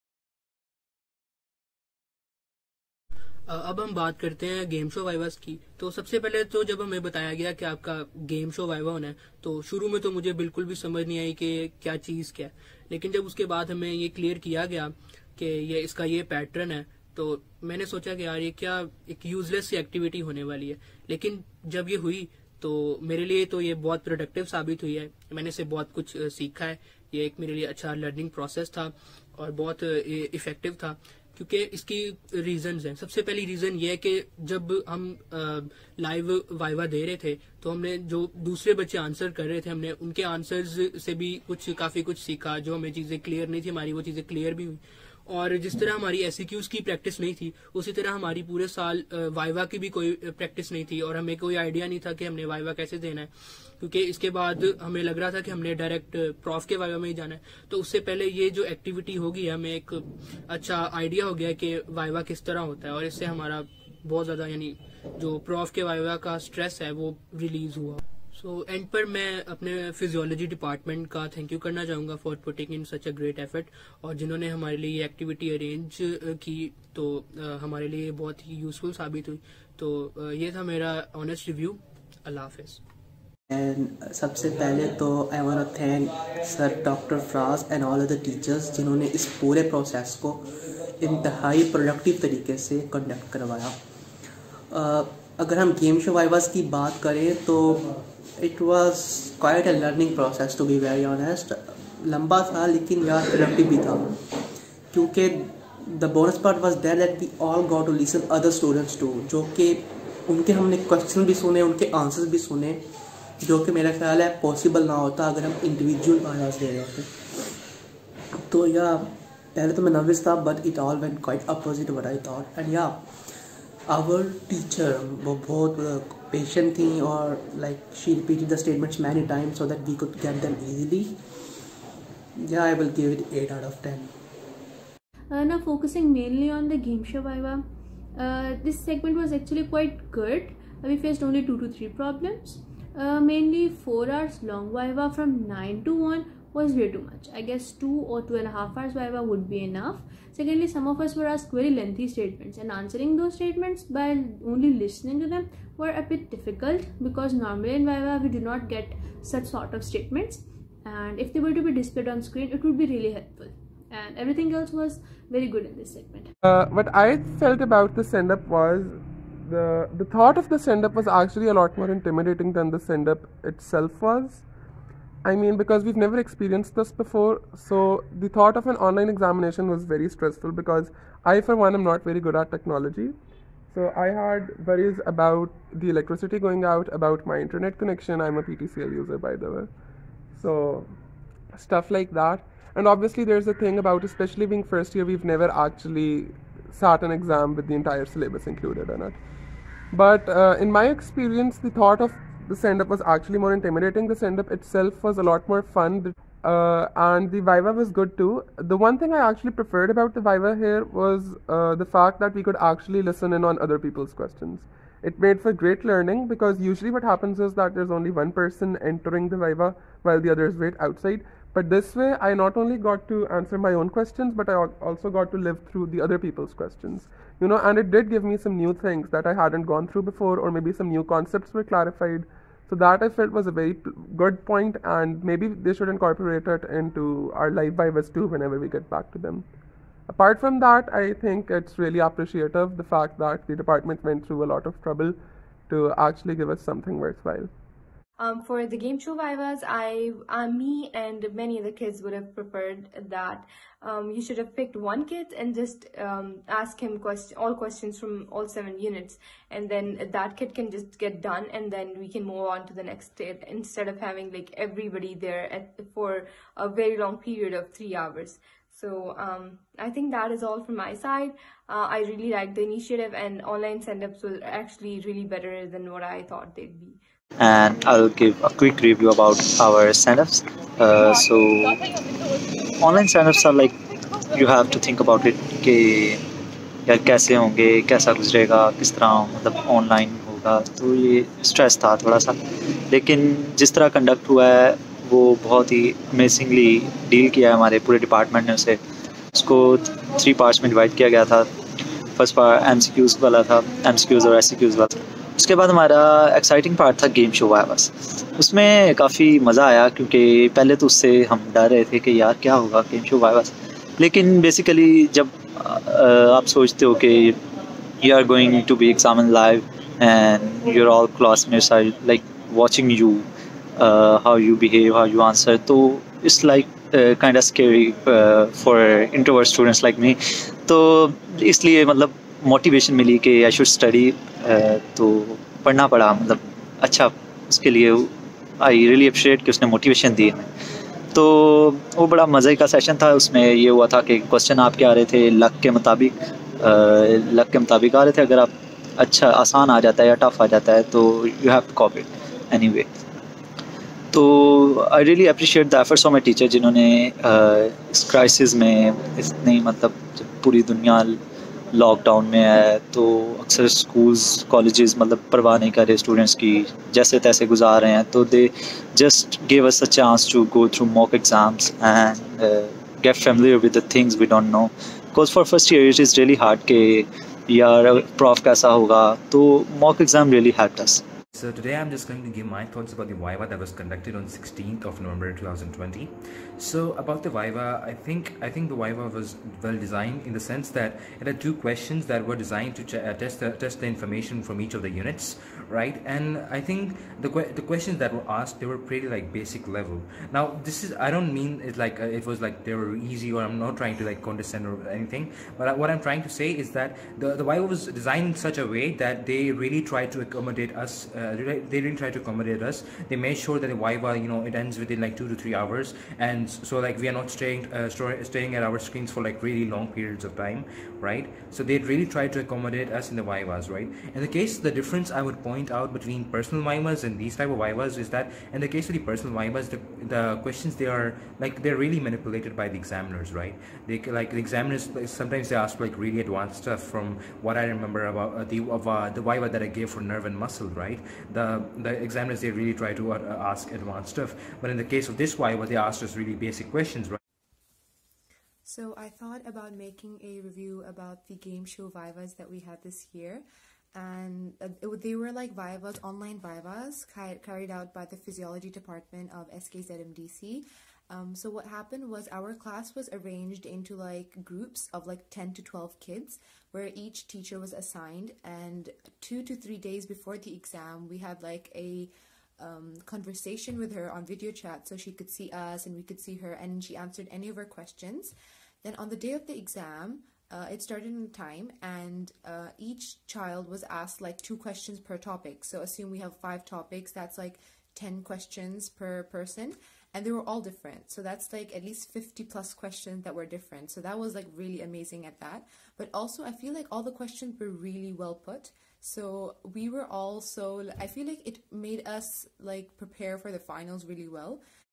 MBBS Students' Assessment: Game Show Viva (GSV) - Student reviews!
We innovated a new assessment tool which addresses some of the classical issues relating to the way vivas were conducted. Lets hear it from the students!